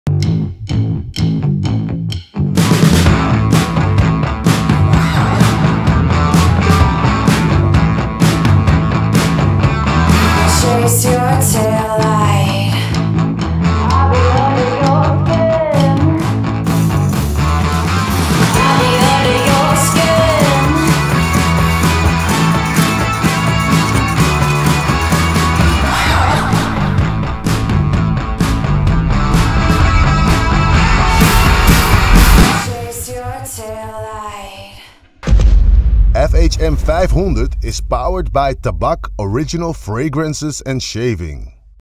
FHM500 2022 is coming...
Oh yes, in samenwerking met onze vrienden van TABAC Fragrances gaan we vanaf volgende week weer los met onze FHM500 campagne! Vanaf 28 april kun je stemmen op jouw favoriet op FHM500.nl. Wie wordt de Mooiste Vrouw van Nederland van 2022? Om je een beetje op te hypen hebben we alvast een lekkere samenvatting van afgelopen jaar gefabriceerd. Stemmen doe je vanaf 28 april op

De FHM500 is powered by TABAC Men's Fragrances & Shaving

🔴⚪️ Volg FHM ook op social media ⚪️🔴

⚪️🔴 Volg TABAC Fragrances ook op social media 🔴⚪️